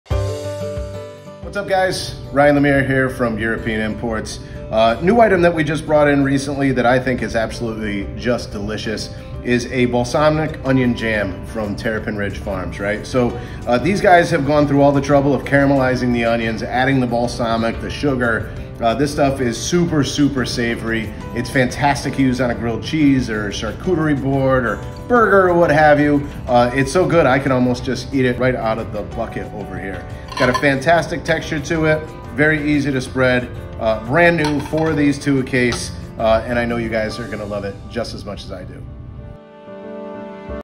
Ryan Spreads Awareness of The Exciting Applications of Balsamic Onion Jam
Meet Terrapin Ridge Farms foodservice size Balsamic Onion Jam! Category Specialist, Ryan, guides us through the delicious, versatile flavors of this kitchen essential. Hear what makes this sensational jam a game-changing ingredient.